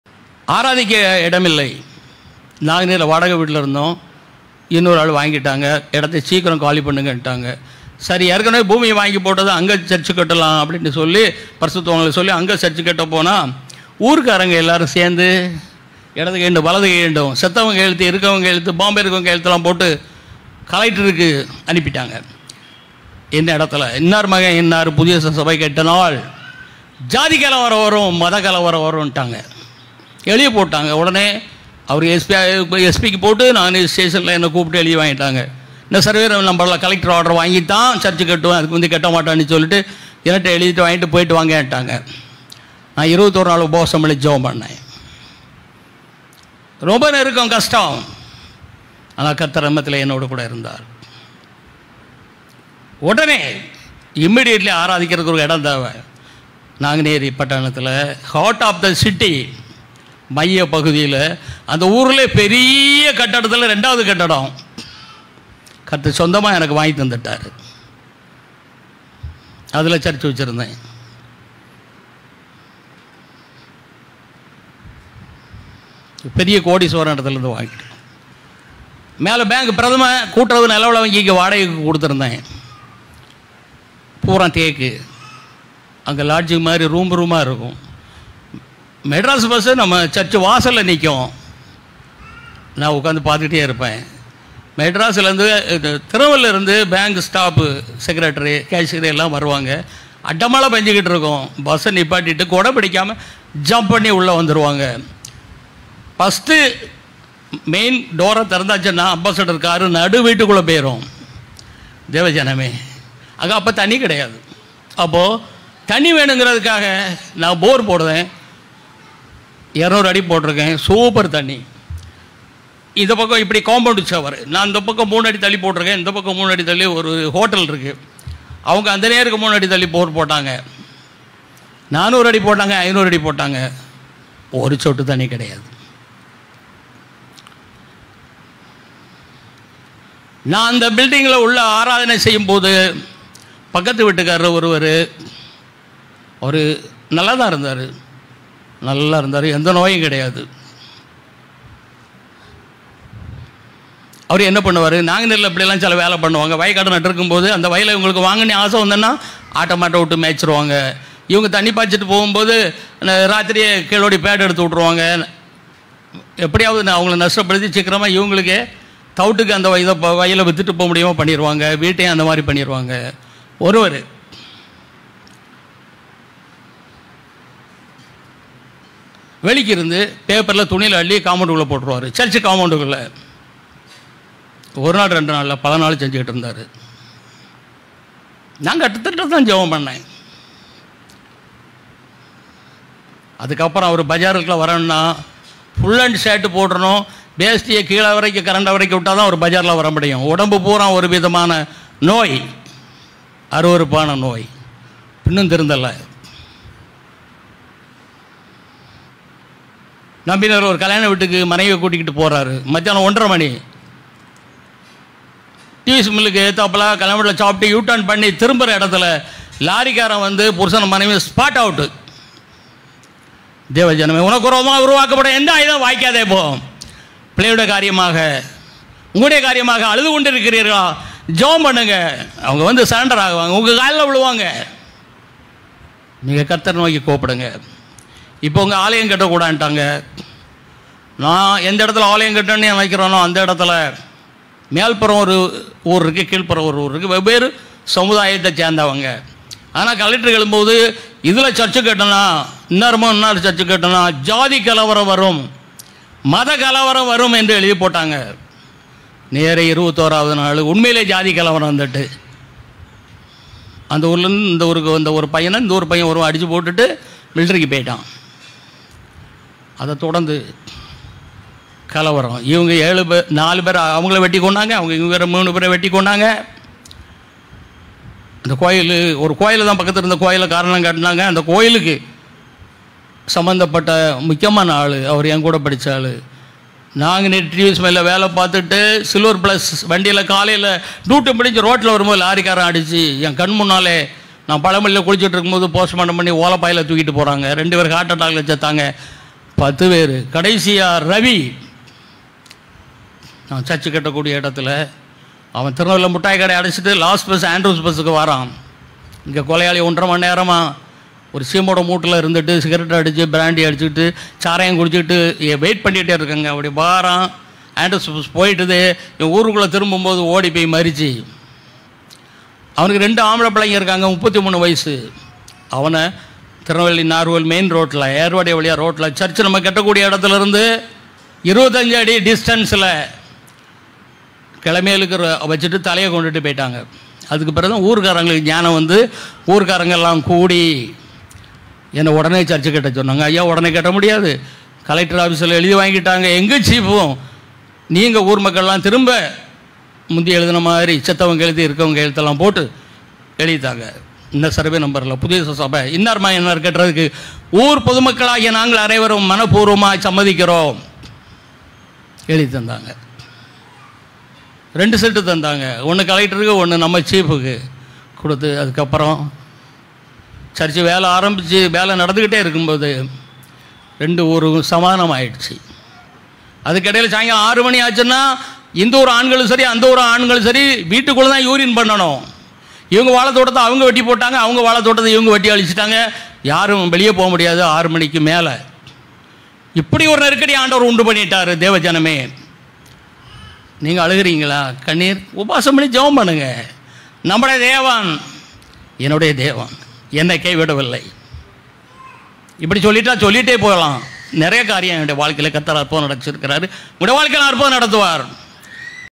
நாங்குநேரி ஊழியம்| NJC Nanguneri| New Jerusalem Church
புதிய எருசலேம் சபை நாங்குநேரி கிளை சபை ஆரம்ப சாட்சி